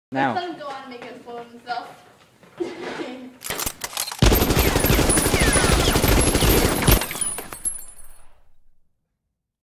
Action Movie FX guns
Cops showed up and decide to shoot my brother Josh... No biggie